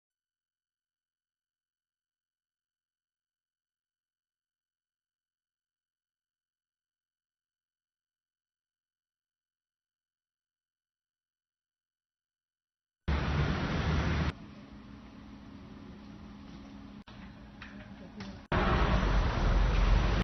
Mainland tourists injured in Taiwan bus accident
About 21 tourists from the Chinese mainland were injured in a bus accident in Kaohsiung city, southeast China's Taiwan.